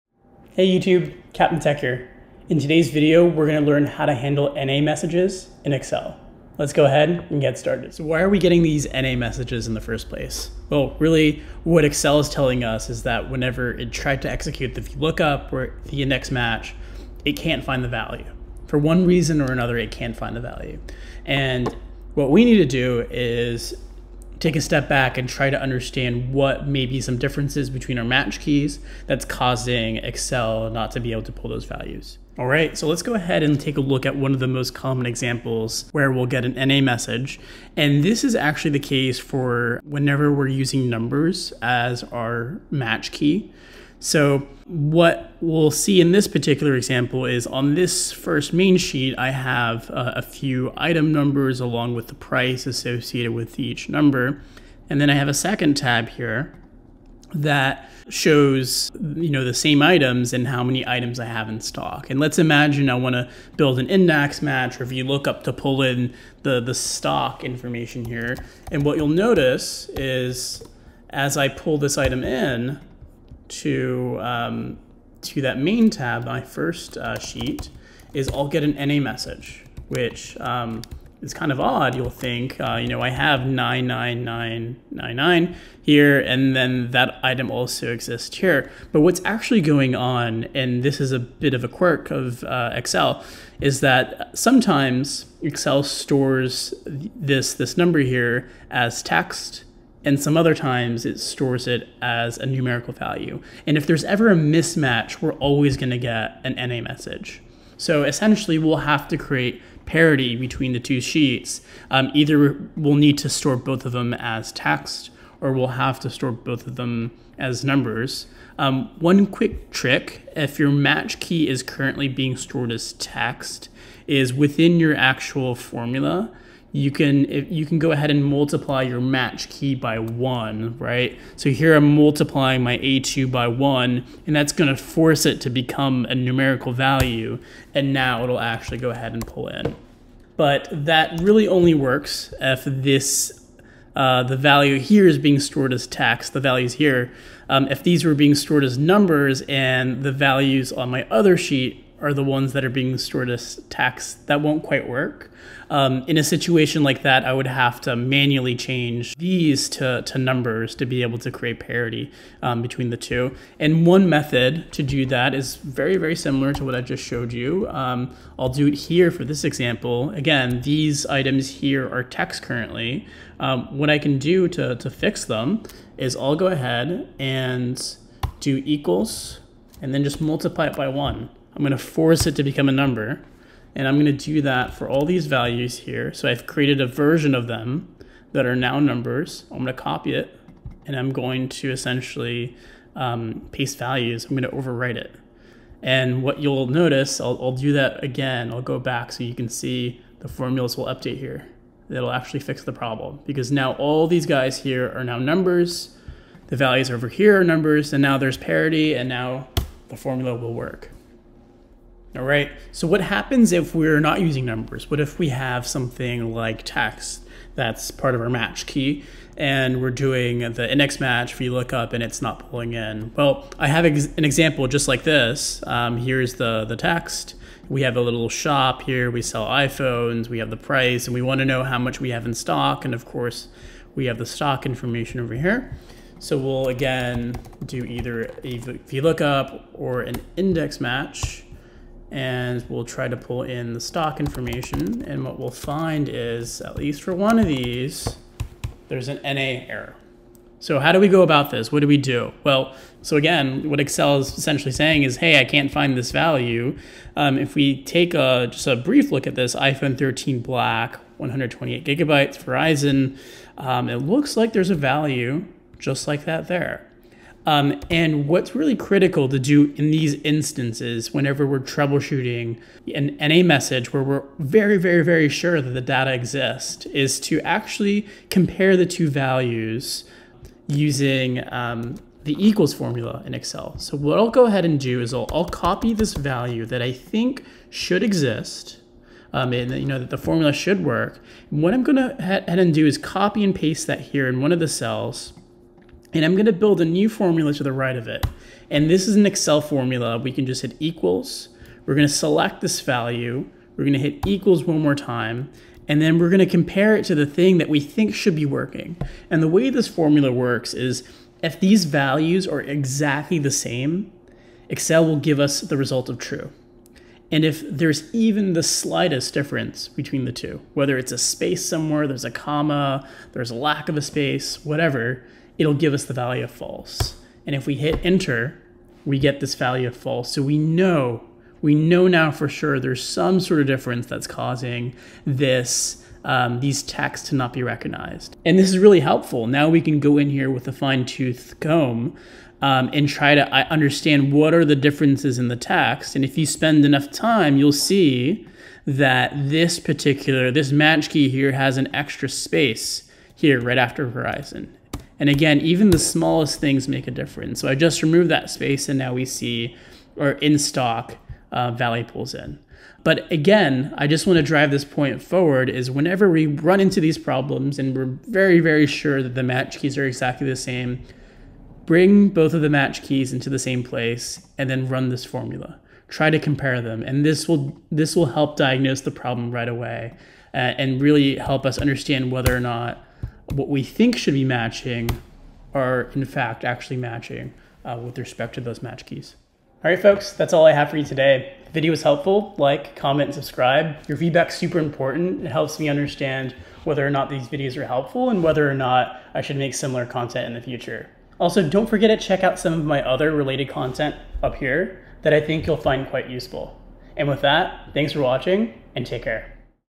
Fixing N/a Vlookup or Index Match Errors in Excel
Stuck on a problem? Schedule a 1-Hour Consulting session with me.

In this video, I will walk you through how to address N/a errors in excel when using a vlookup or index match

OVERVIEW
0:00 - Intro
0:08 - Why Are We Getting an NA?
0:33 - Using a Number as a Match Key
3:31 - Using Text as a Match Key
6:29 - Like, Comment, & Subscribe